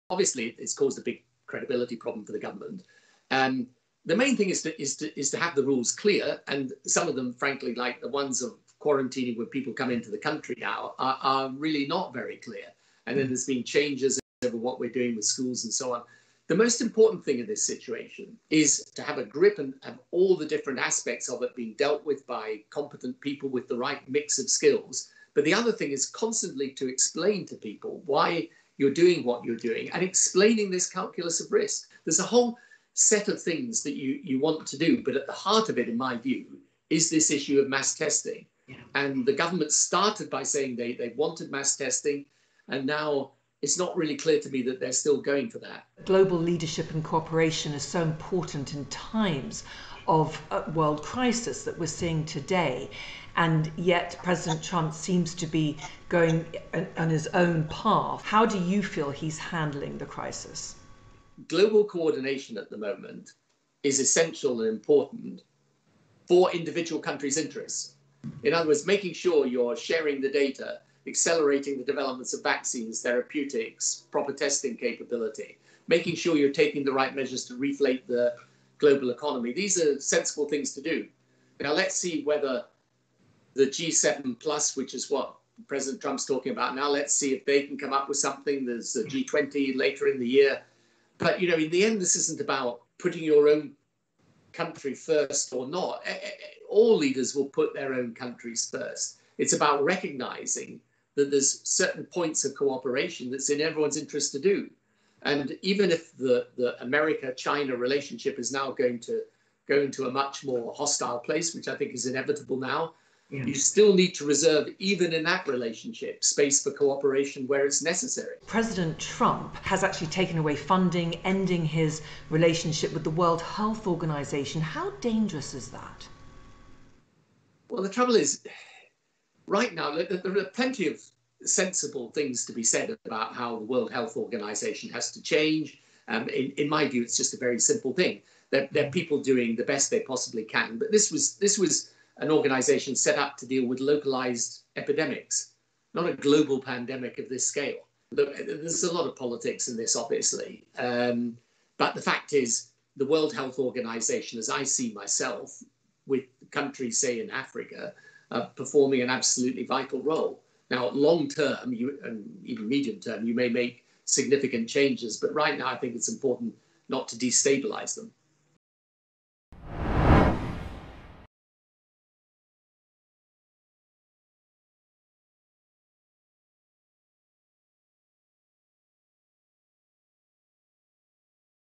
Tony Blair on UK government's Covid-19 rules and 'credibility problem'
CNBC's Tania Bryer speaks exclusively to the former U.K. Prime Minister and Founder and Executive Chairman of The Tony Blair Institute for Global Change. In a wide ranging interview, Blair says some of the rules put in place by the U.K. government in response to the Covid-19 global crisis are not very clear, and that the political situation involving Prime Minister Boris Johnson's senior aide, Dominic Cummings, who faced pressure to resign after allegedly breaking government lockdown rules, had caused 'a big credibility problem for the government'.